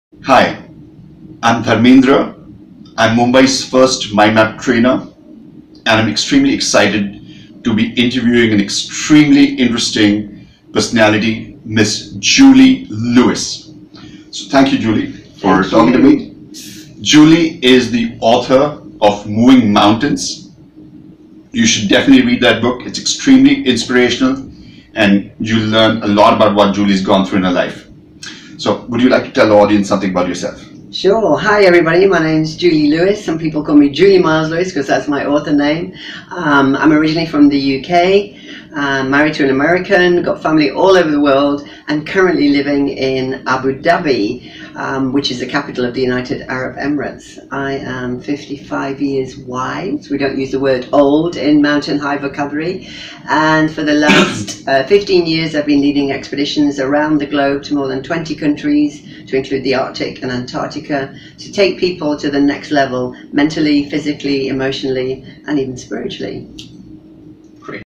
Dharmendra Rai Interviews Julie Lewis
Dharmendra Rai , Mumbai 's First Mind Map Trainer , interviews Julie Lewis , Author - Mountain High Me & Professional Speaker .